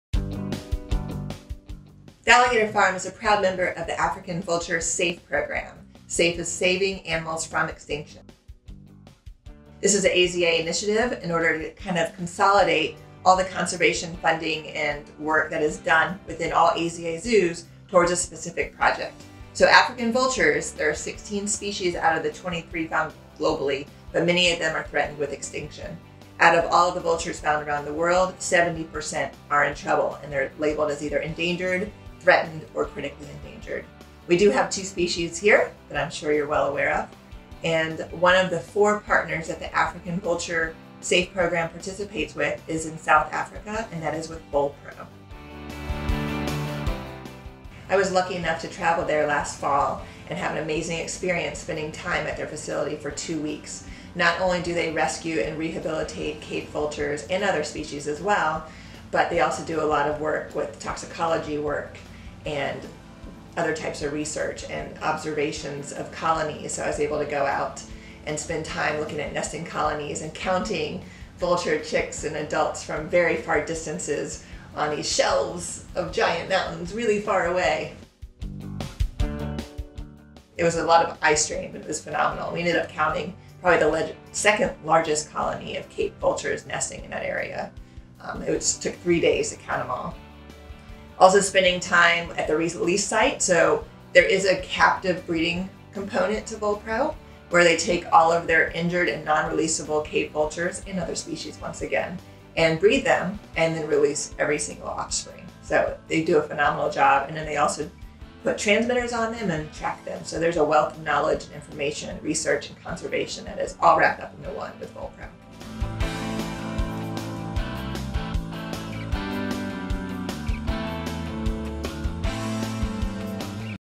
Saving Our Scavengers: Cape Vultures
Cape Vultures are one of 2 endangered species of vultures we have at the St. Augustine Alligator Farm, and in 2019, our General Curator made a trip to South Africa where they're found.

She volunteered at a vulture conservation facility called VulPro, and she is going to tell us about her amazing experience.

Cape Vultures are threatened with poisoning, hunted for traditional medicine, and collisions with powerlines.

International Vulture Awareness Day is celebrated on the first Saturday of September every year, to promote the conservation of nature's clean-up crew.